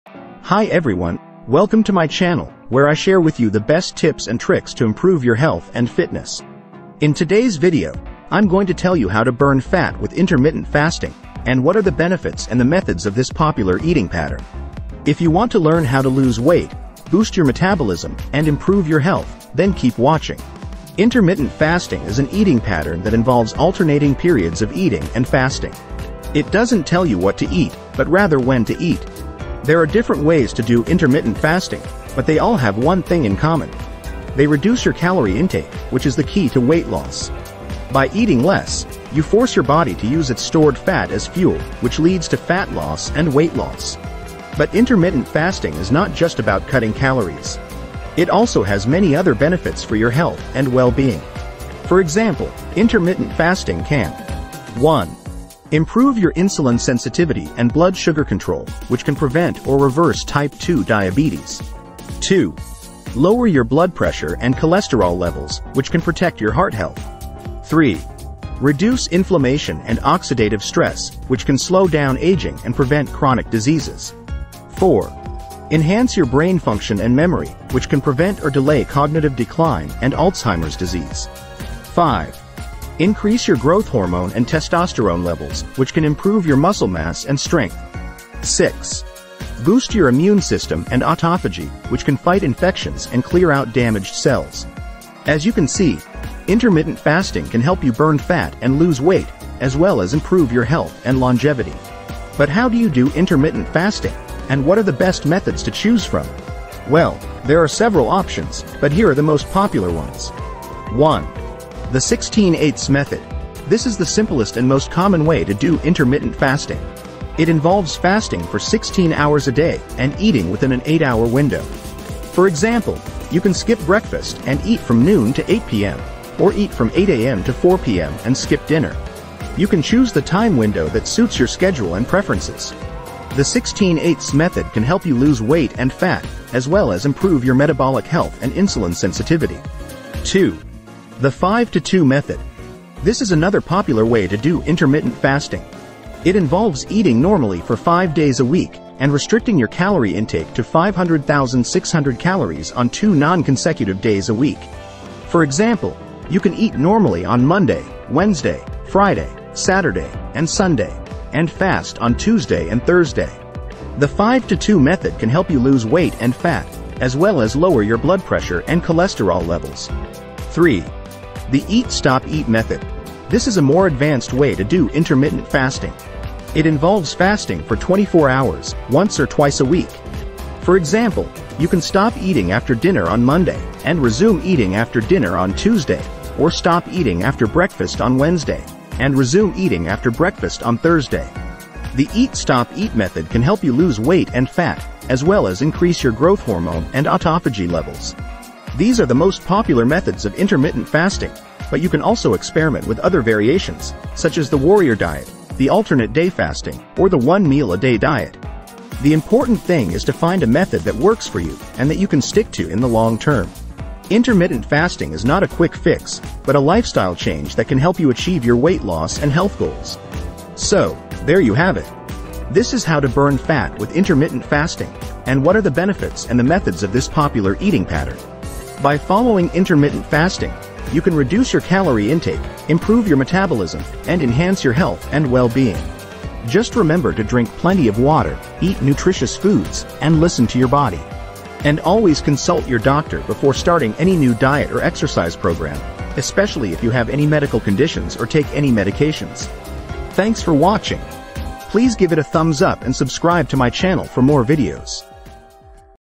Intermittent Fasting For Beginners: How To Burn Fat For Better Health
A Step-by-Step Beginner's Guide to Intermittent Fasting: How to Burn Fat Effectively" - If you're new to intermittent fasting and want to explore the best strategies for burning fat, this video is a must-watch! We provide easy-to-follow steps, essential tips, and even sample meal plans to kickstart your fat-burning journey and achieve better health through intermittent fasting.